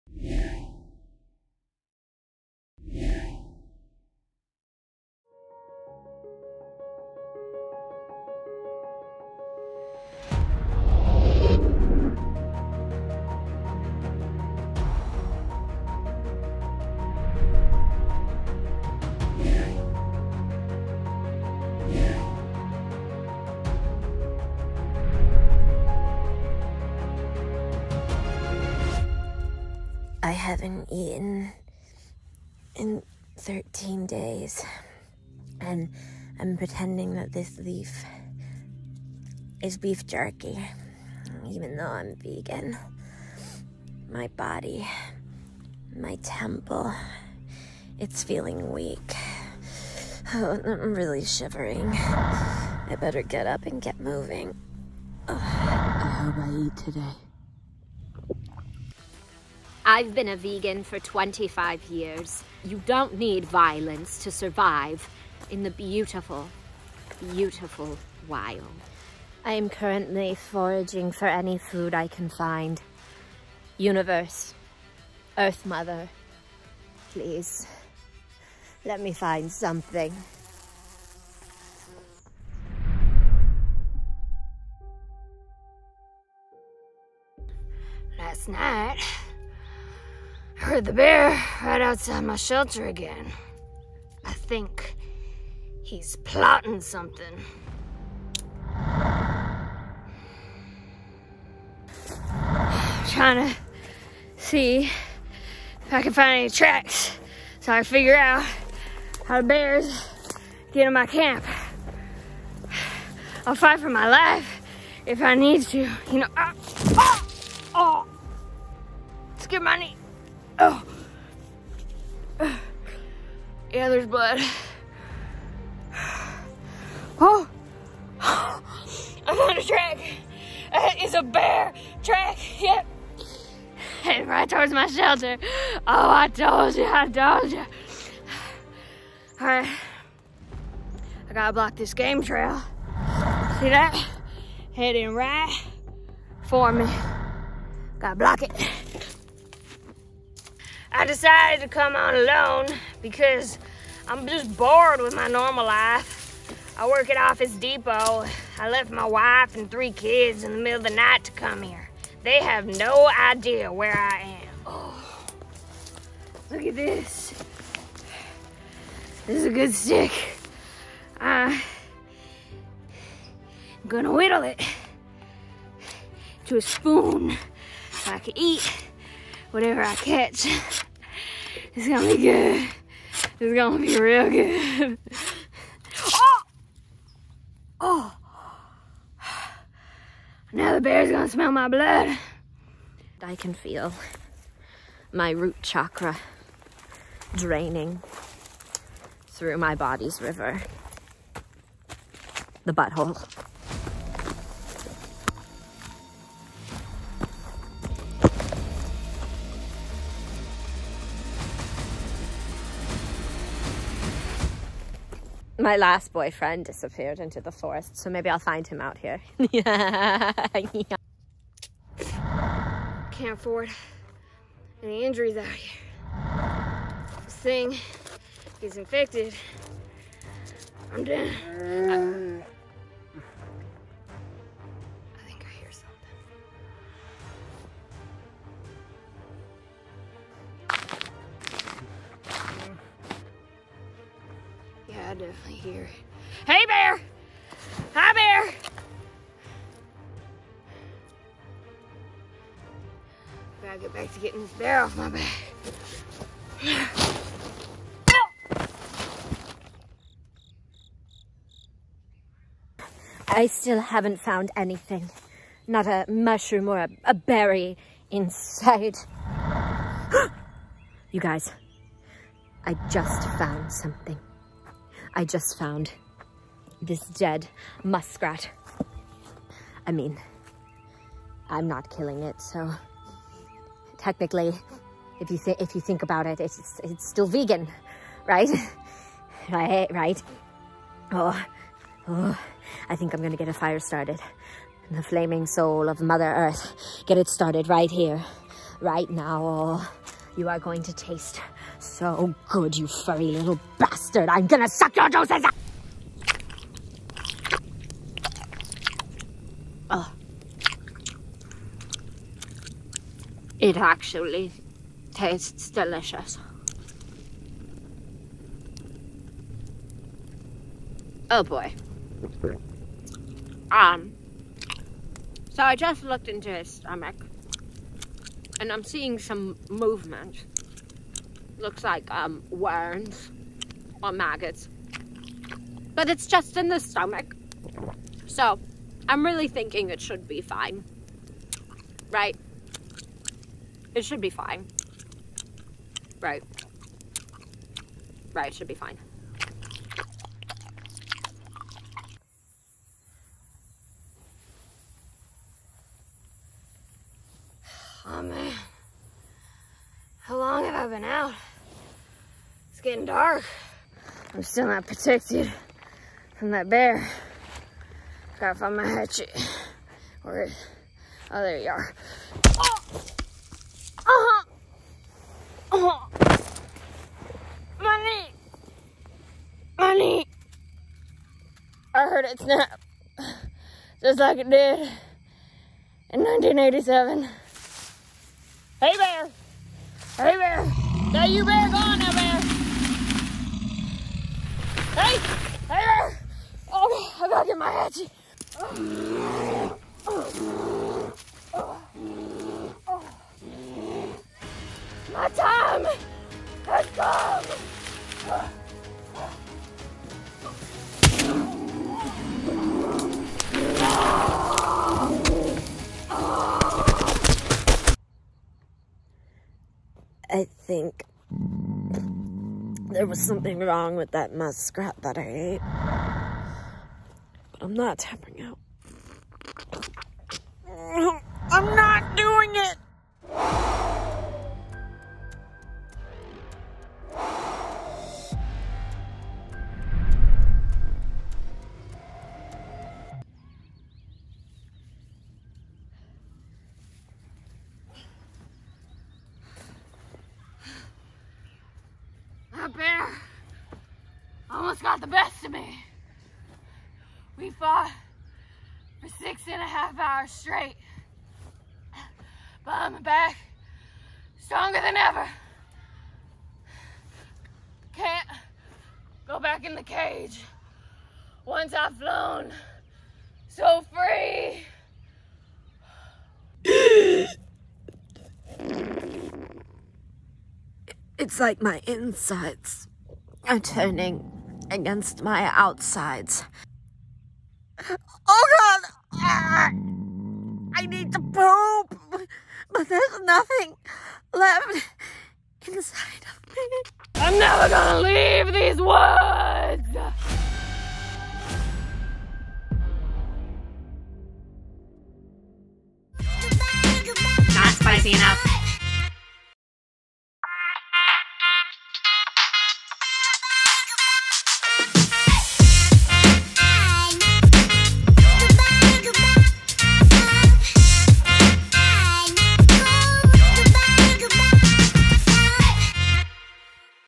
ALONE - EXTREME SURVIVAL Every Episode Ever | Reality Television History Channel Parody
Alone, in the woods, with nothing but yourself. Would you do it for $500,000? These two endured starvation, bears, and food poisoning for the money, and you get to watch their suffering for free. Who will win? You'll have to endure the wild with them to find out.

Featuring:
Cece Lucero-Trask & Tuesday Grant

Directed by: Cece Lucero-Trask & Tuesday Grant
Written by: Cece Lucero-Trask

Production:
Not Spicy Enough

Editing:
Tuesday Grant

Thanks to:
Marc Rosenzweig
Zephyr Entertainment
Envato Elements
Shutterstock

Special Thanks:
John Irvine & Taylor Grant
Katherine & Matt Trask